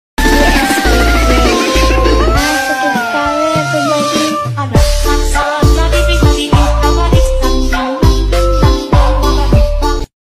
non lo fatto io ma dovete solo leggere in descrizione
apro un con set dove voi mi fate vedere il vostro oc

e io faccio un meme edit su di voi